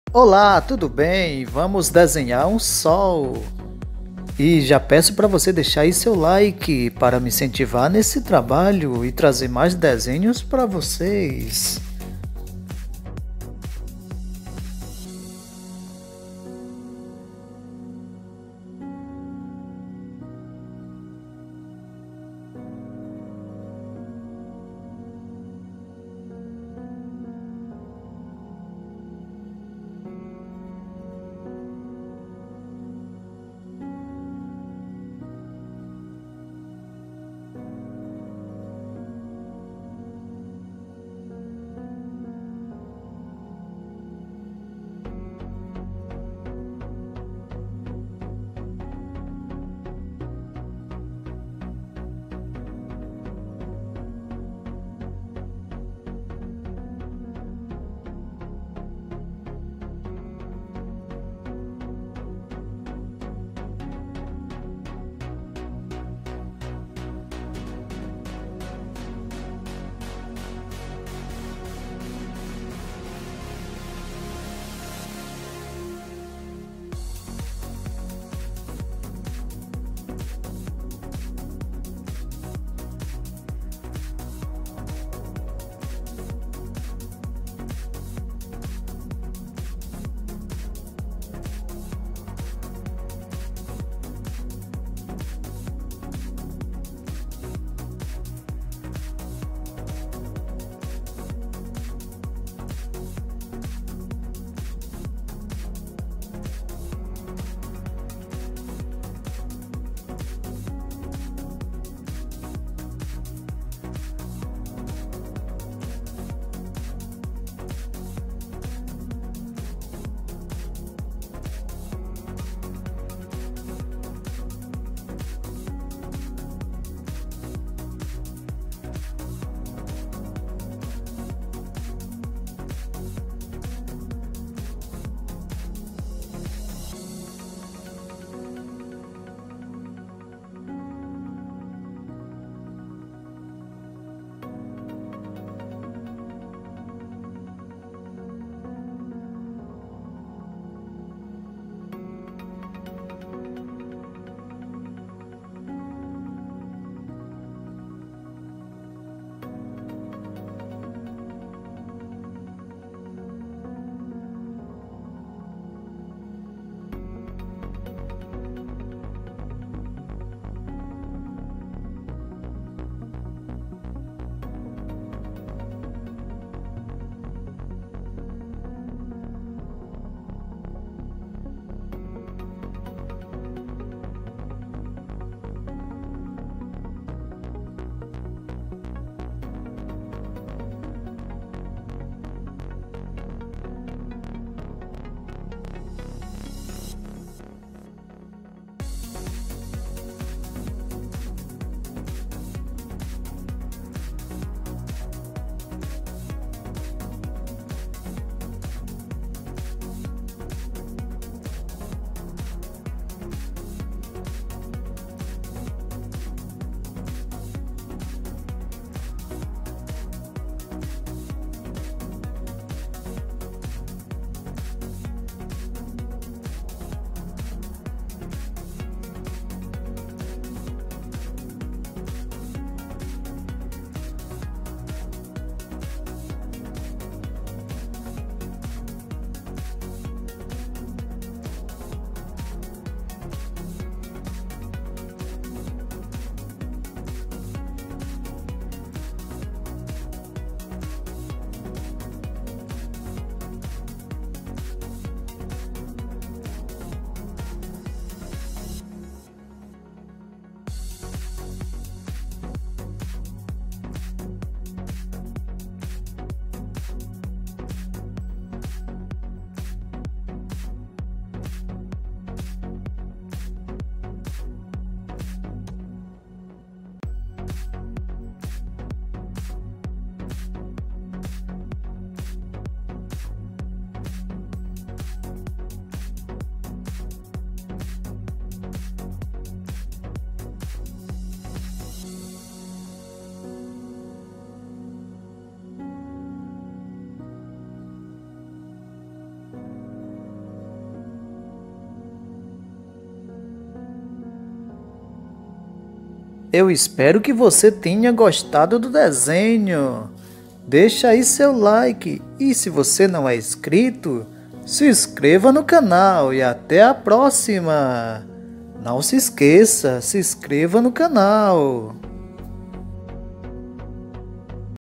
Como desenhar um Sol passo a passo / how to draw a sun step by step
veja como desenhar fácil um Sol passo a passo, É um desenho de sol fácil de desenhar e bonito. Acredito que você vai gostar desse desenho de sol

how to draw a sun for beginners